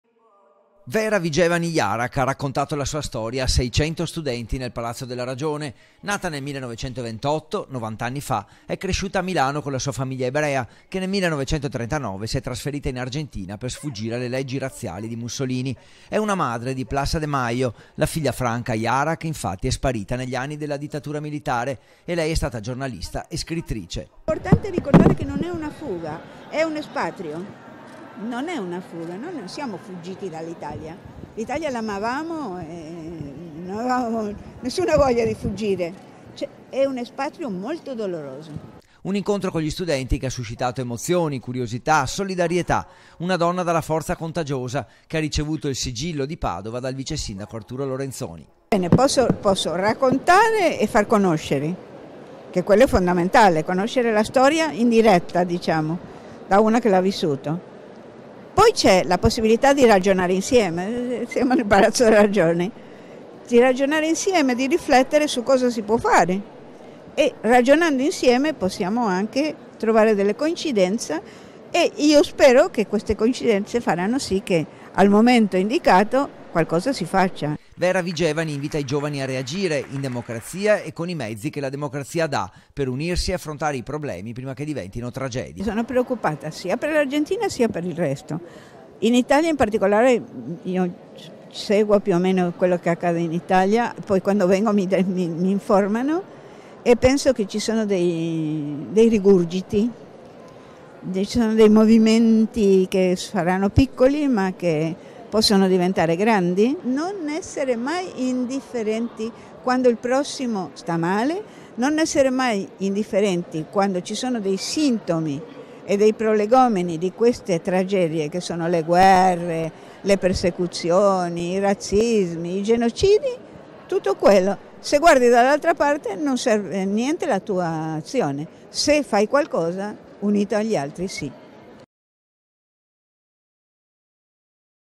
TG (15/10/2018) - EMIGRATA IN ARGENTINA 80 ANNI FA, RACCONTA LA SHOAH
TG (lunedì 15 ottobre 2018) - Vera Vigevani Jarach ha raccontato la sua storia a 600 studenti nel Palazzo della Ragione. Nata nel 1928, 90 anni fa, è cresciuta a Milano con la sua famiglia ebrea, che nel 1939 si è trasferita in Argentina per sfuggire alle leggi razziali di Mussolini. E' una madre di Plaza de Mayo, la figlia Franca Jarach è sparita negli anni della dittatura militare, ed è stata giornalista e scrittrice. Un incontro con gli studenti che ha suscitato emozioni, curiosità, solidarietà. Una donna dalla forza contagiosa, che ha ricevuto il sigillo della città dal vicesindaco Arturo Lorenzoni.
Vera Vigevani invita i giovani a reagire, in democrazia e con i mezzi che la democrazia dà per unirsi e affrontare i problemi prima che diventino tragedie.Sono italiana è ho il diritto e il dovere di raccontare le mie preoccupazioni, e poi un consiglio ai giovani.